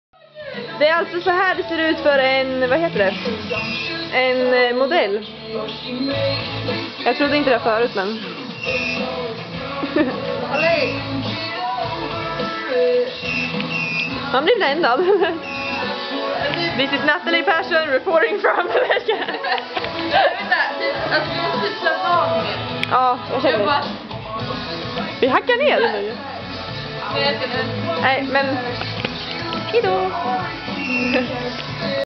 Mimi och Nathalie Chillar i studion
En snabb film från fotostudion i skolan.